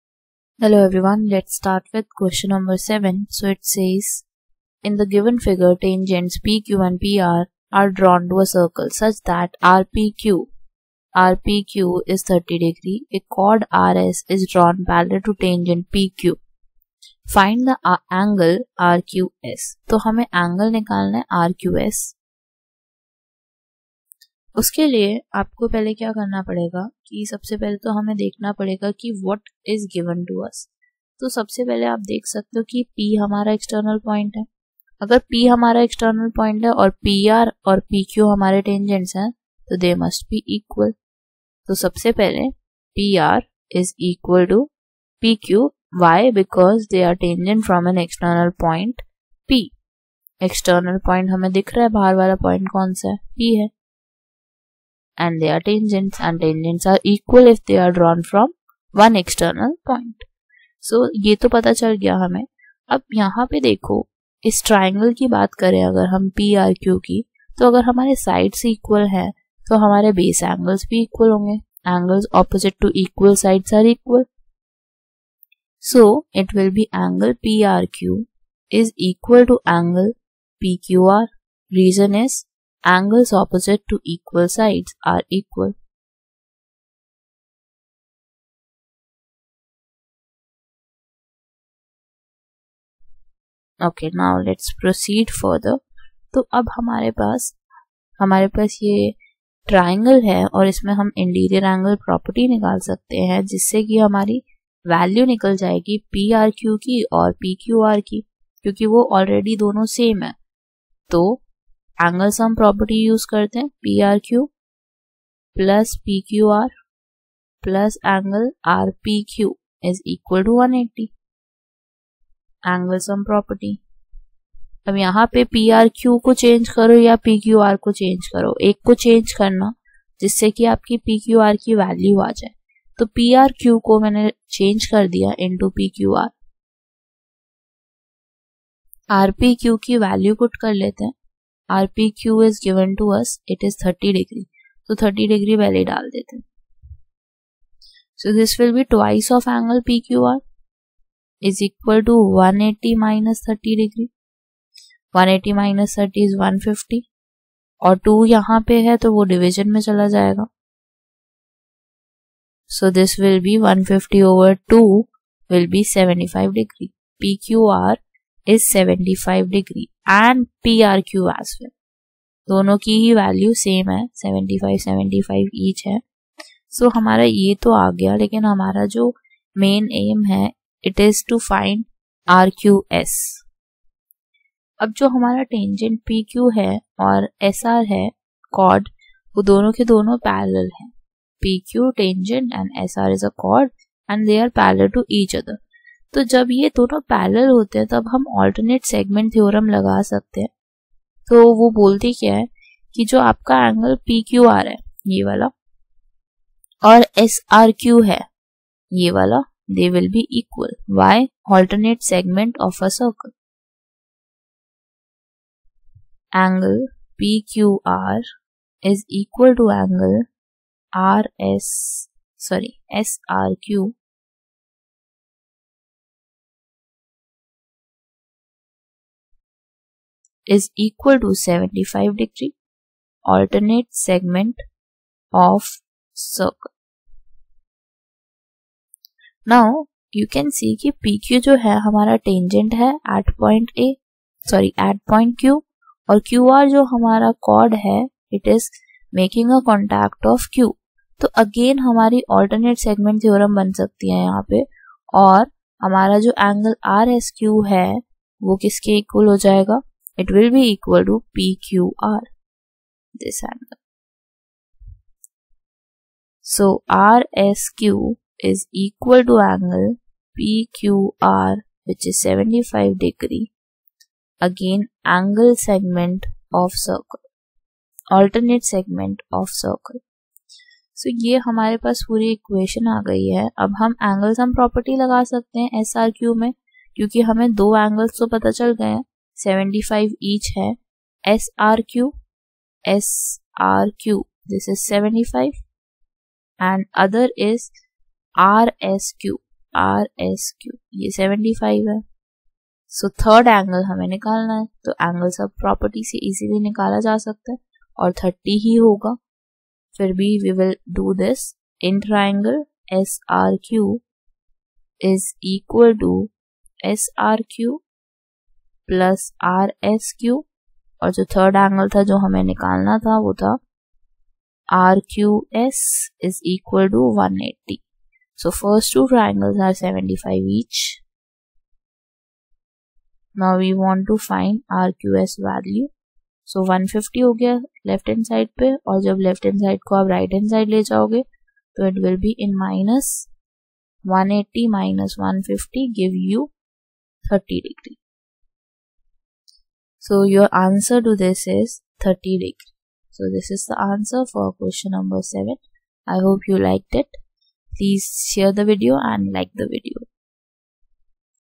7. In the given figure, tangents PQ and PR are drawn to a circle such that ∠RPQ = 30° A chord RS is
7. In the given figure, tangents PQ and PR are drawn to a circle such that ∠RPQ = 30°  A chord RS is